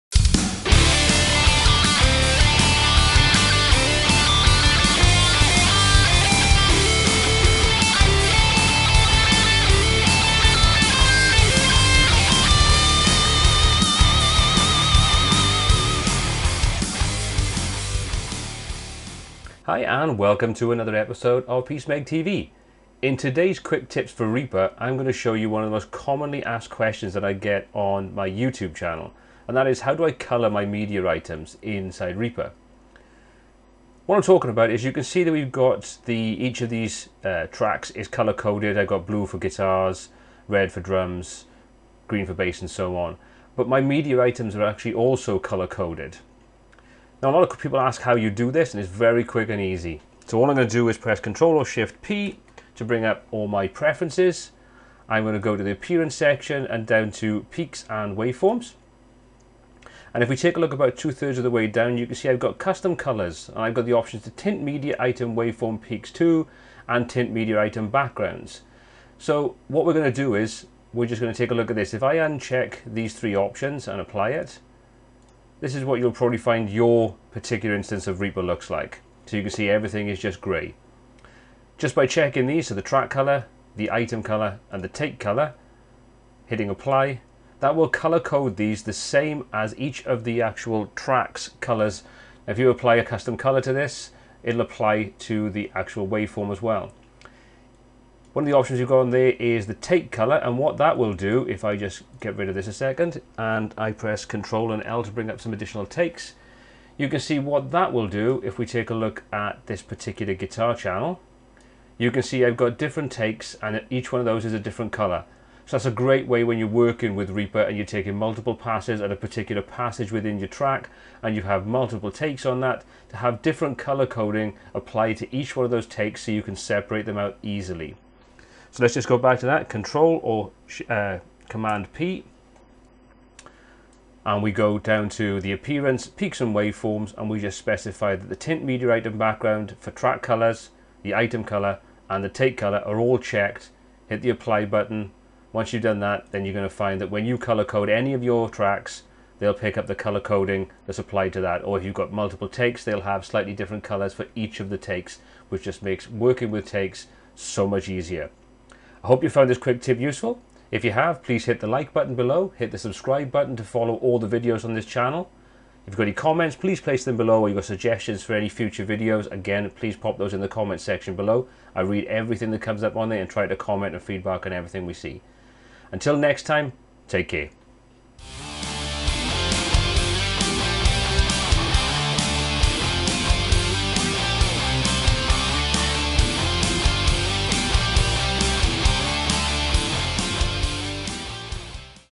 Reaper Quick Tips: Colour Coding Your Media
One of the most commonly asked questions on my YouTube Channel is 'How do I make my media colour coded like yours?'

This quick tip for Cockos Reaper DAW will show you how to achieve this colour coding quickly and easily.